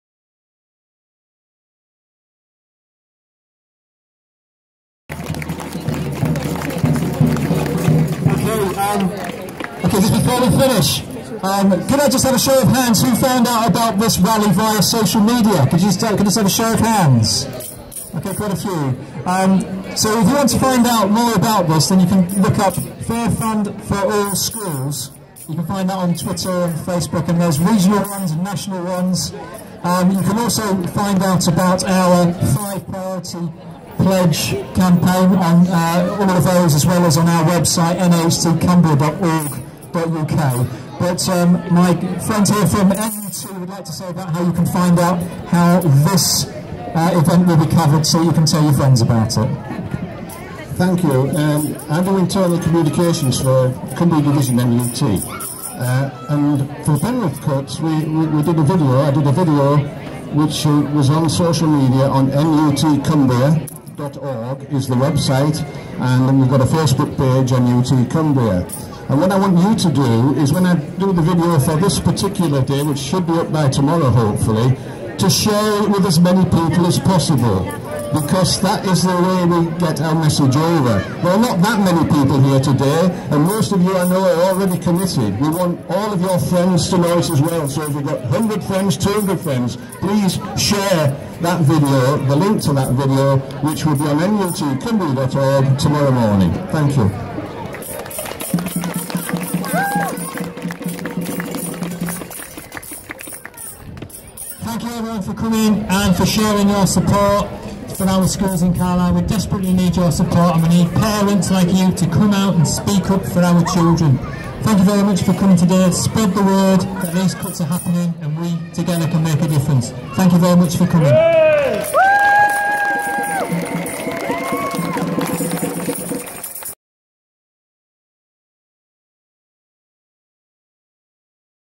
Carlisle Rally: Closing Remarks - Fair Funding For Schools [11]
Things are heating up! More and more people are realising how much damage will be done to our children and our communities if the proposed cuts to school funding go ahead.

My family attended another rally, this time held in Carlisle, Cumbria. It was attended by the community, school faculty, MP candidates (but of course not the local Tory MP!) teachers, headteachers and of course, PARENTS.

The whole event was organised by a local parent: Janice Bell, whom we owe our thanks for getting this rally off the ground and getting word out there. It was wonderful to see so many turn out on a Sunday afternoon in Carlisle to show their support and raise their voice against the proposed cuts.

The 8th of June is getting closer and closer, we have to show those in power that we will NOT stand idly by and let them decimate our children's future!

All kids are of value, and their talents and skills matter.

Please, speak to your local political candidates and see where they stand on the issue of school funding. And go out there and vote on June the 8th!

We want Fair Funding for All Schools!

If you decide to try this design too, why not share it with me? I'd love to see it!